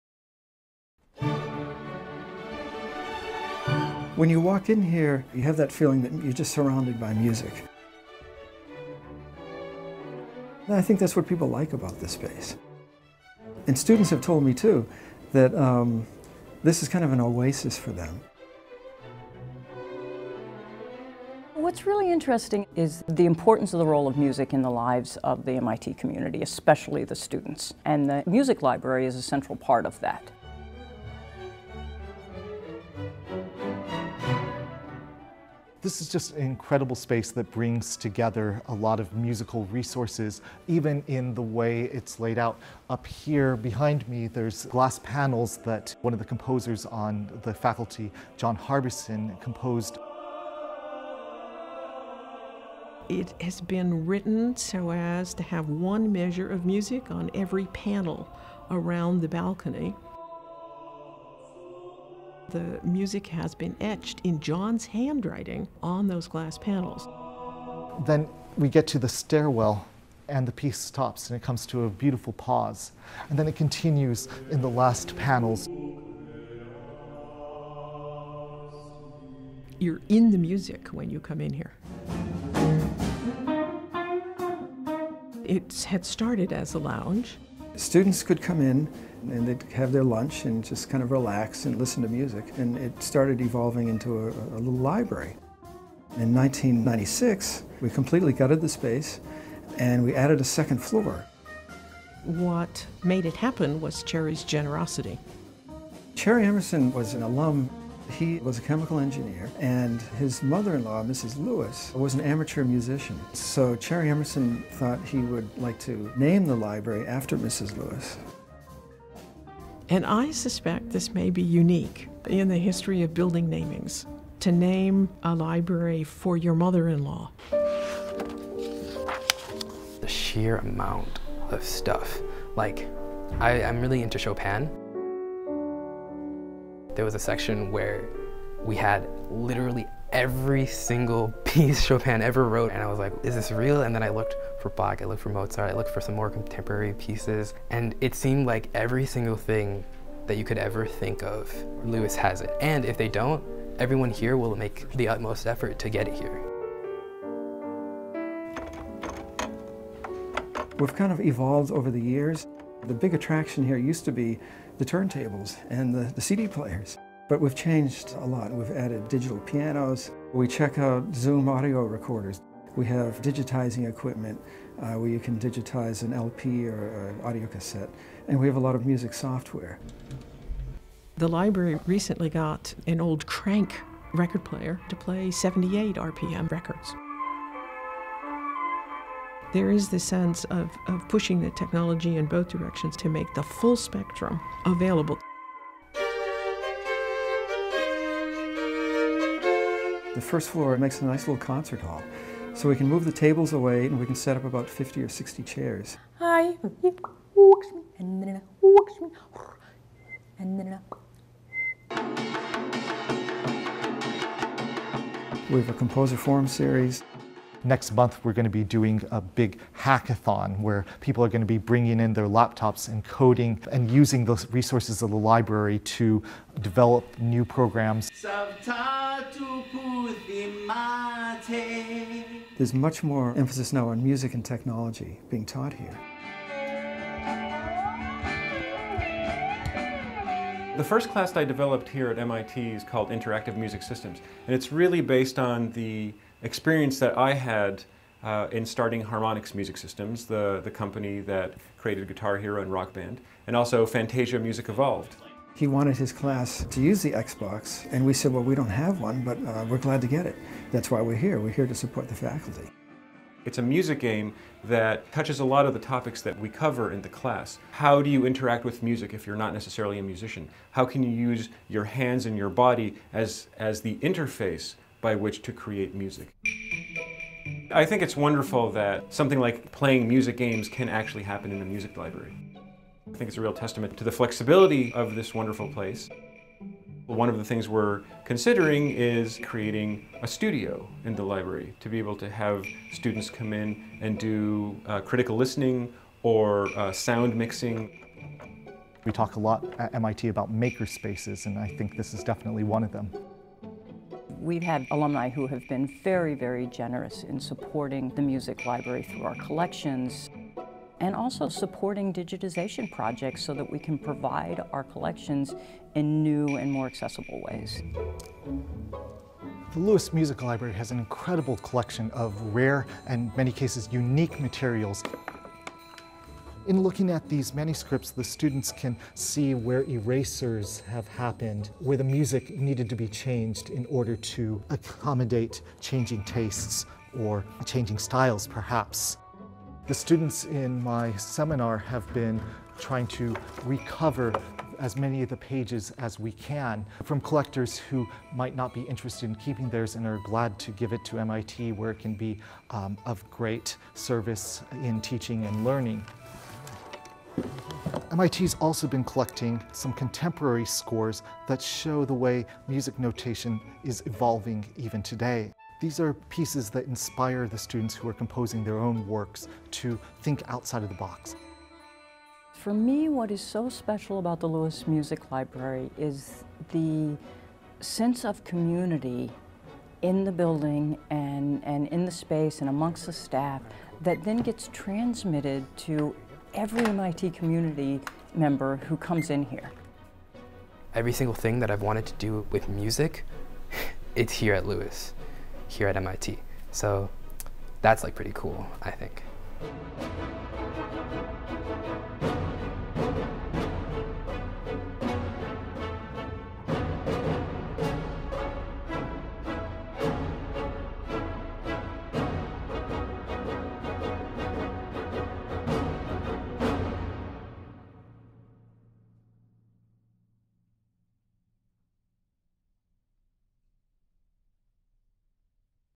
The Rosalind Denny Lewis Music Library: Celebrating 20 Years
MIT’s Rosalind Denny Lewis Music Library holds a special place in the hearts and minds of students, faculty, and alumni alike. On the 20th anniversary of the library’s renovation and dedication, we celebrate two decades of supporting teaching, learning, and the love of music across the MIT community.